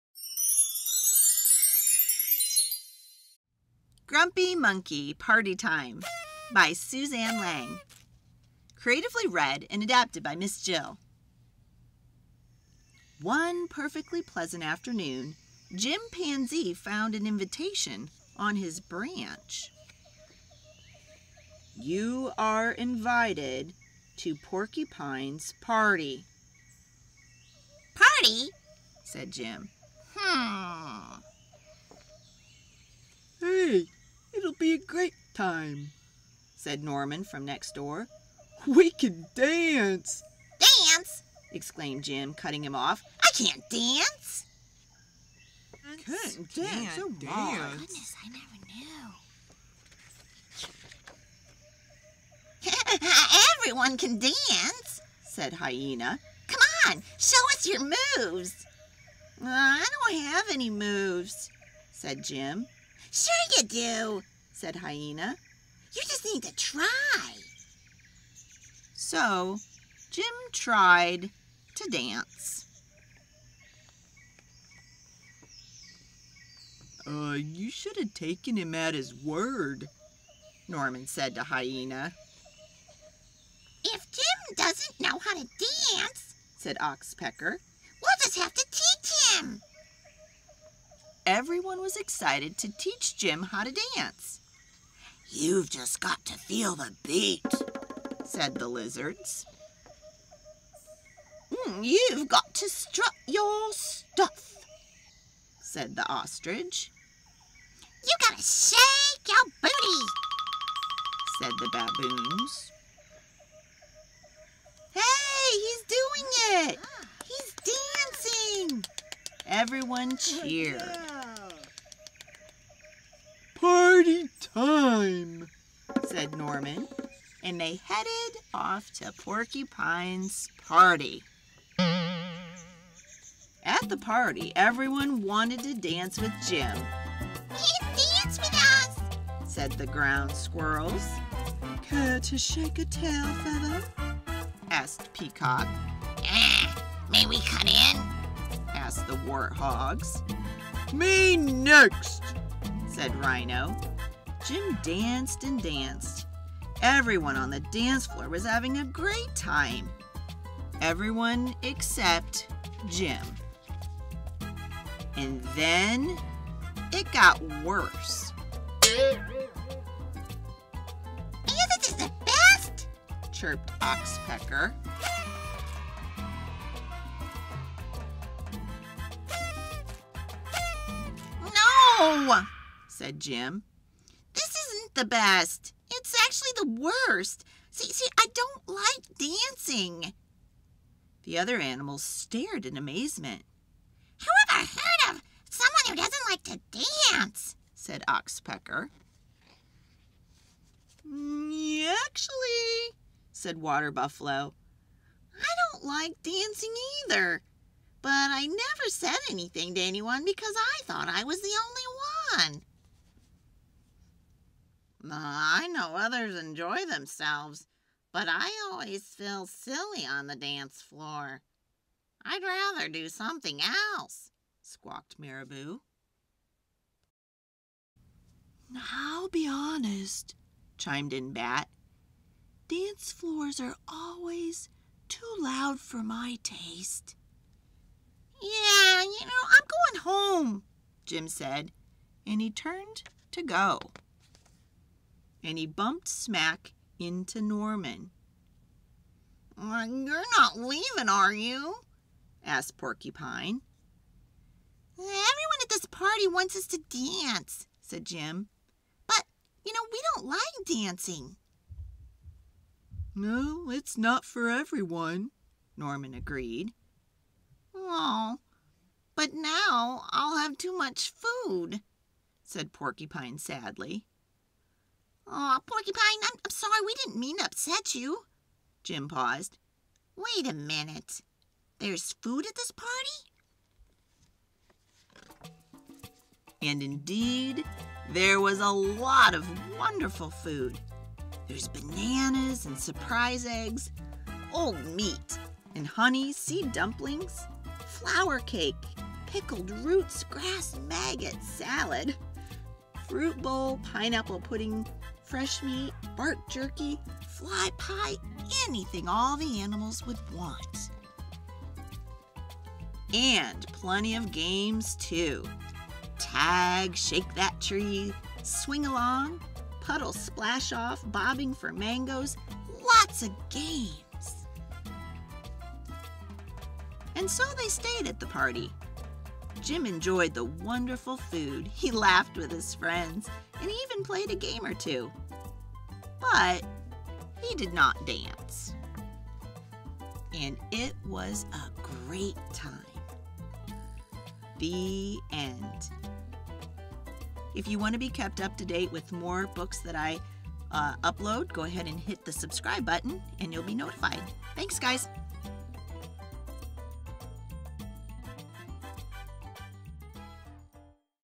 🐵Grumpy Monkey Party Time (Read Aloud books for children) Sound Effects
🍄Monkey is invited to a party but he doesn't want to dance! Please join us for a dramatic read of Grumpy Monkey Party Time by Suzanne Lang read by Miss Jill. This is the second book.